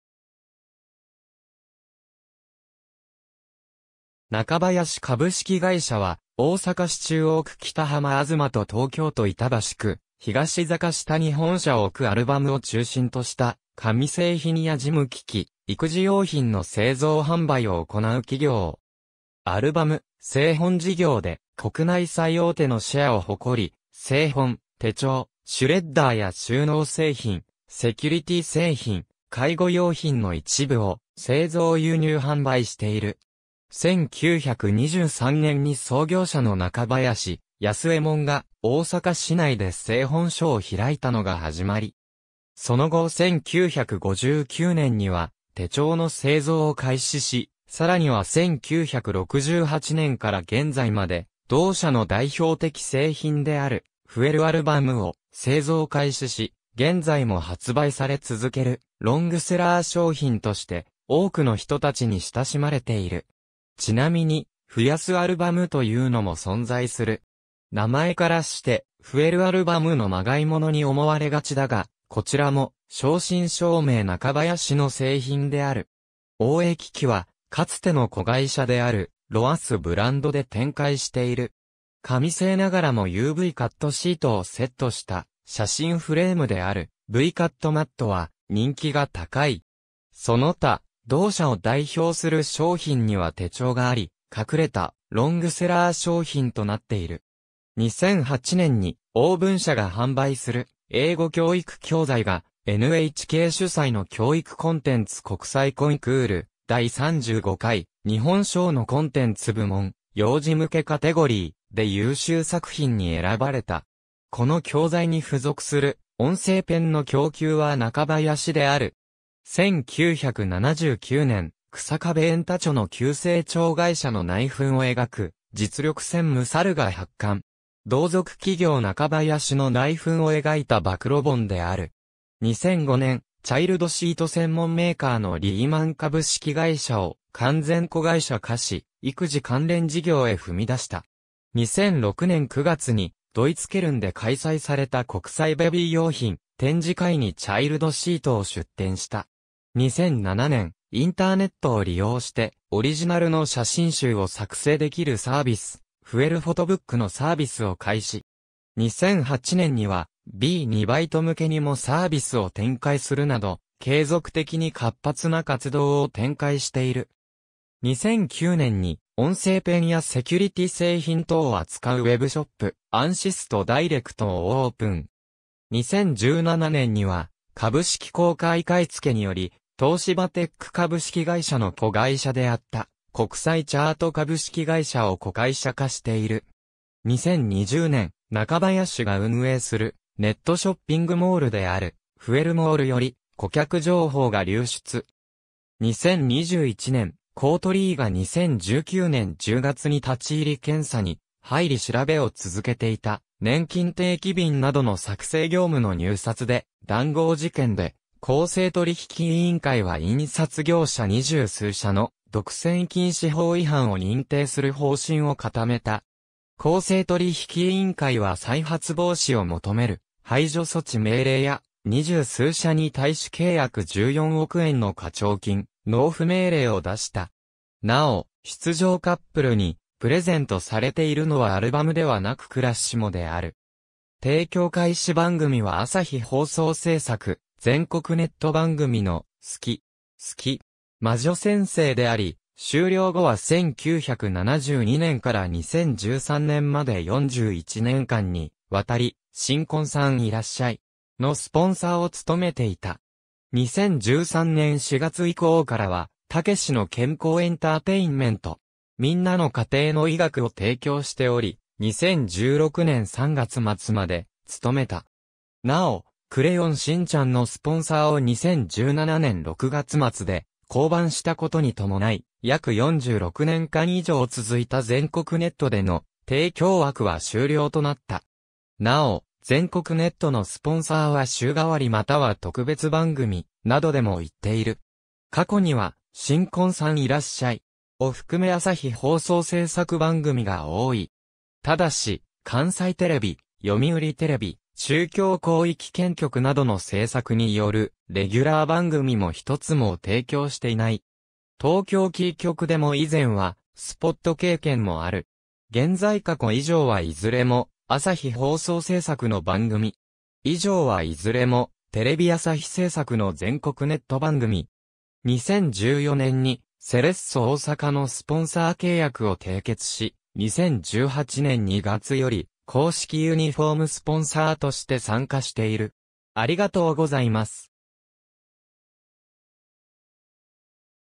ナカバヤシ
ナカバヤシ株式会社（Nakabayashi Co.,Ltd.）は、大阪市中央区北浜東と東京都板橋区東坂下に本社を置くアルバムを中心とした紙製品や事務機器、育児用品の製造販売を行う企業。
アルバム・製本事業で国内最大手のシェアを誇り、製本・手帳・シュレッダーや収納製品・セキュリティ製品、介護用品の一部を製造輸入販売している。
1923年（大正12年）に創業者の中林安右衛門が大阪市内で製本所を開いたのが始まり。
その後1959年（昭和34年）には手帳の製造を開始し、さらには1968年（昭和43年）から現在まで同社の代表的製品である「フエルアルバム」を製造開始し、現在も発売され続けるロングセラー商品として、多くの人たちに親しまれている。
ちなみに「フヤスアルバム」というのも存在する。
名前からしてフエルアルバムのまがい物に思われがちだが、こちらも正真正銘ナカバヤシの製品である。
OA機器はかつての子会社である「ロアス」ブランドで展開している。
紙製ながらもUVカットシートをセットした写真フレームであるVカットマットは人気が高い。
その他、同社を代表する商品には手帳があり、隠れたロングセラー商品となっている。
2008年に旺文社が販売する英語教育教材がNHK主催の教育コンテンツ国際コンクール・第35回「日本賞」のコンテンツ部門【幼児向けカテゴリー】で優秀作品に選ばれた。
この教材に付属する音声ペンの供給はナカバヤシである。
1979年、草壁 焔太著の急成長会社の内紛を描く『実力専務去る』が発刊。
同族企業ナカバヤシの内紛を描いた暴露本である。
2005年、チャイルドシート専門メーカーのリーマン株式会社を完全子会社化し、育児関連事業へ踏み出した。
2006年9月にドイツ・ケルンで開催された国際ベビー用品展示会にチャイルドシートを出展した。
2007年、インターネットを利用してオリジナルの写真集を作成できるサービス「フエルフォトブック」のサービスを開始。
2008年にはB2B向けにもサービスを展開するなど、継続的に活発な活動を展開している。
2009年に音声ペンやセキュリティ製品等を扱うwebショップANSISTダイレクトをオープン（後、webショップはIT部門へ引き継ぐ形で ANSISTダイレクトは閉鎖）。
2017年には株式公開買付けにより、東芝テック株式会社の子会社であった国際チャート株式会社を子会社化している。
2020年、ナカバヤシが運営するネットショッピングモールである「フエルモール」より顧客情報が流出（流出した可能性がある情報：顧客情報最大12万件。）。
2021年、公取委が2019年10月に立ち入り検査に入り調べを続けていた「ねんきん定期便」などの作成業務の入札で談合事件で、公正取引委員会は印刷業者二十数社の独占禁止法違反（不当な取引制限）を認定する方針を固めた。
　　公正取引委員会は再発防止を求める排除措置命令や、二十数社に対し計約14億円の課徴金納付命令を出した（ナカバヤシの課徴金は3億1071万円。）。
なお、出場カップルにプレゼントされているのはアルバムではなくクラッシモである。
提供開始番組は朝日放送制作・全国ネット番組の『好き! すき!! 魔女先生』であり、終了後は1972年（昭和47年）から2013年（平成25年）まで41年間にわたり『新婚さんいらっしゃい!』のスポンサーを務めていた（なお、後継スポンサーは、ペティオ）。
2013年4月以降からは『たけしの健康エンターテインメント!みんなの家庭の医学』を提供しており、2016年3月末まで務めた。
なお、『クレヨンしんちゃん』のスポンサーを2017年6月末で降板した事に伴い、約46年間以上続いた全国ネットでの提供枠は終了となった。
なお、全国ネットのスポンサーは週替わり又は特別番組などでも行っている。
過去には『新婚さんいらっしゃい!』を含め朝日放送制作番組が多い。
ただし、関西テレビ、読売テレビ、中京広域圏局などの制作によるレギュラー番組も1つも提供していない（スペシャル番組には提供経験はある）。
東京キー局でも以前はスポット経験もある。
現在 過去 以上はいずれも、朝日放送（ABCテレビ）制作の番組。
以上はいずれも、テレビ朝日制作の全国ネット番組。
2014年にセレッソ大阪のスポンサー契約を締結し、2018年2月より公式ユニフォーム（パンツ）スポ...